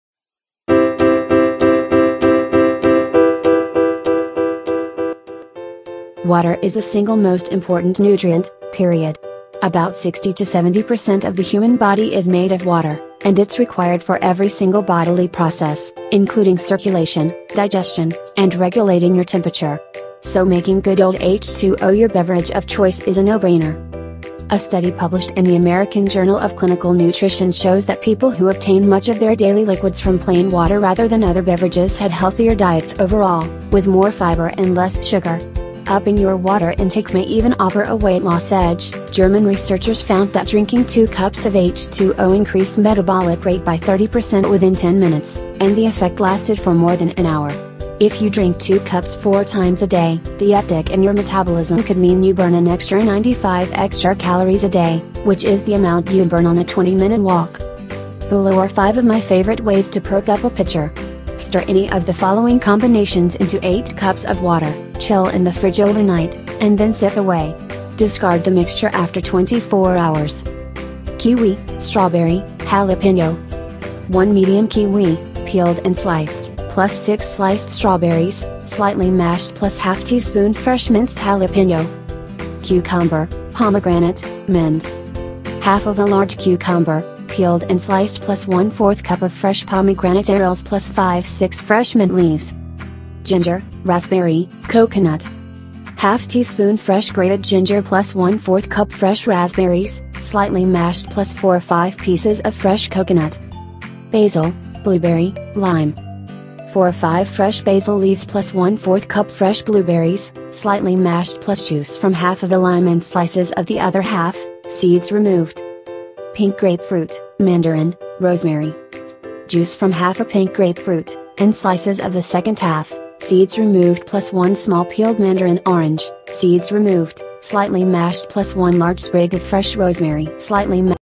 5 Infused Water Recipes That Will Make Your H20 Much Tastier and Even Healthier | Nutrition Pass
5 Infused Water Recipes That Will Make Your H20 Much Tastier and Even Healthier | Nutrition Pass